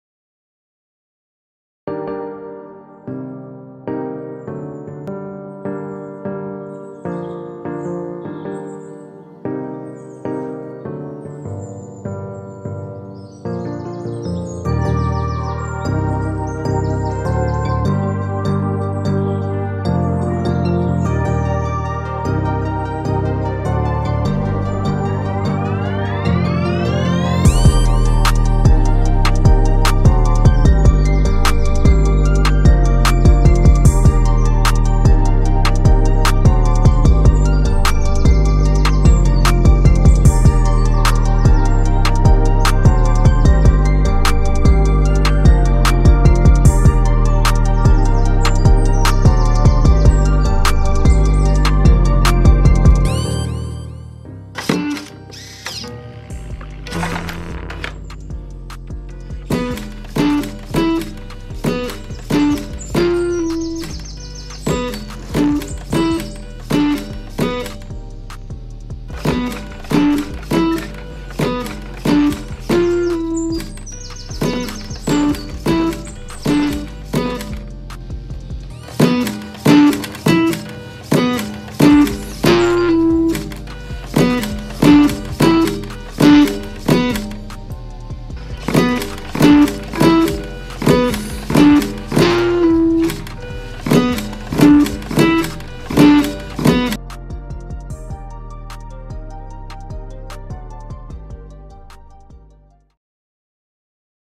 Uke - guitar robot made from an HP Printer
Built from scratch, this machine can play any full size guitar with ease.

Sourced from a couple HP printers, the slider rail is used with an L298N motor controller and an Arduino Uno to move the carriage to position.

A servo is used to press the strings down and an ultrasonic sensor allows for precise...ya ok...movement along the neck of the guitar.